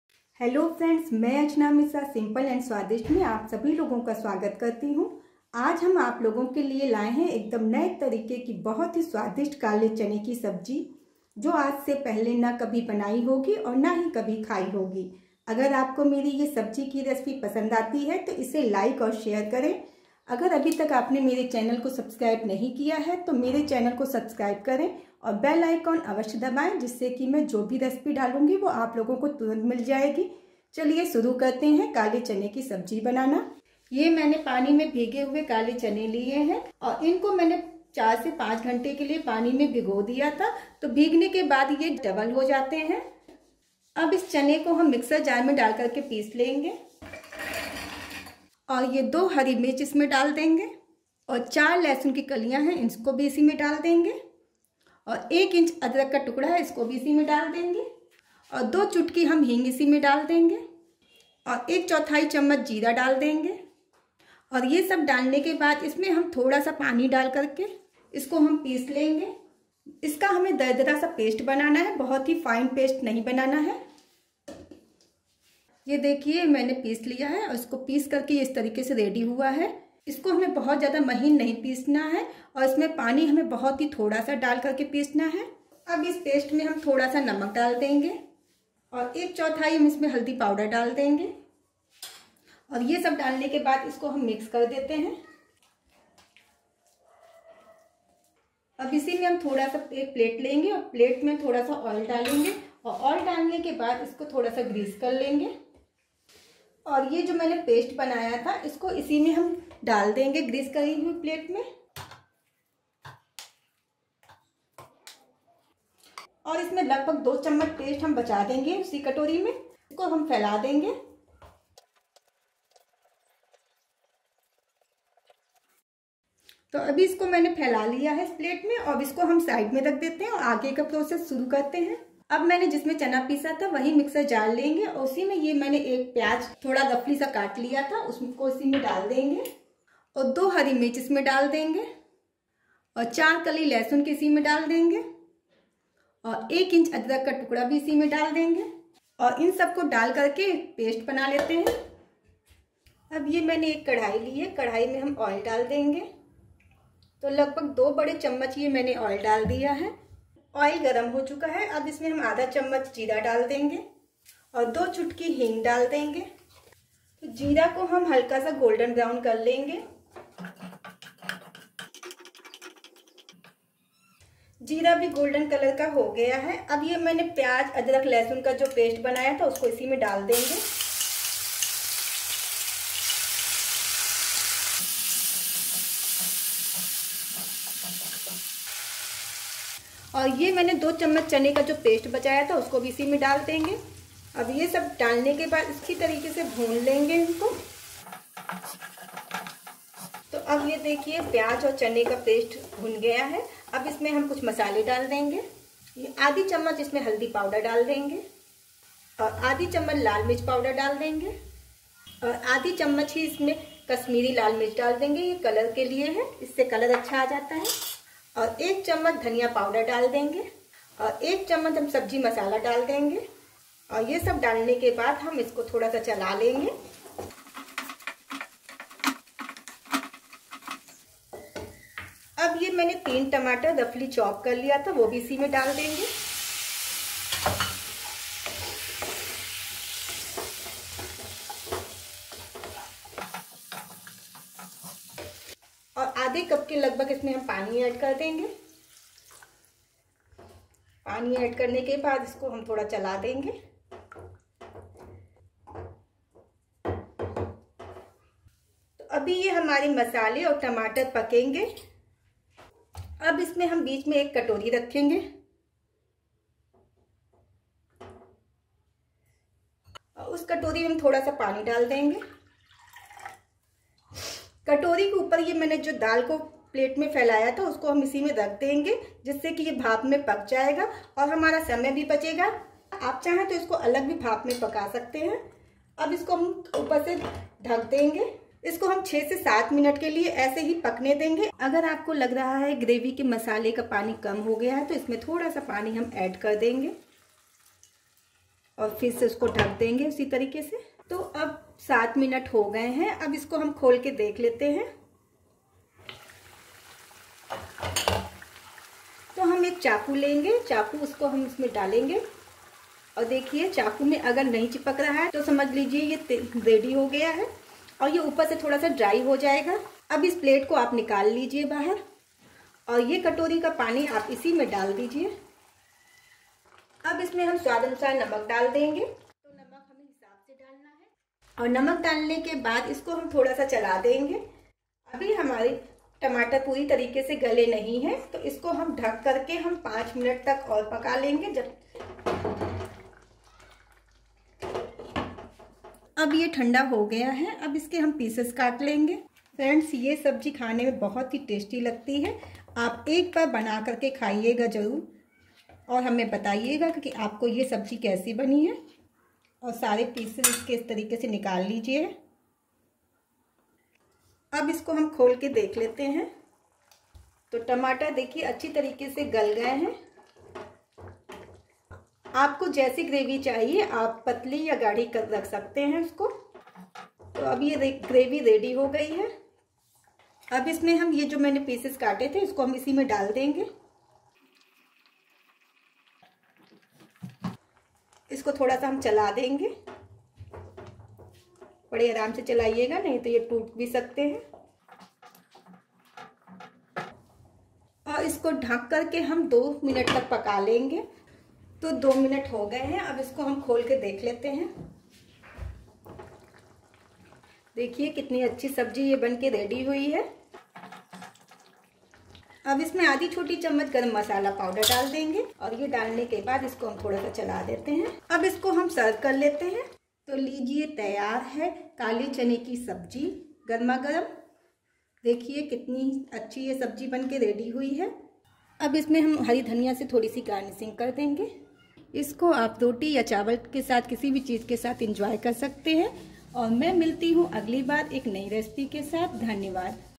काले चने की सब्जी नए तरीके से बनाएंगे तो पुराने सभी तरीके भूल जाएंगे | kale chane ki sabji
Some of My recipe List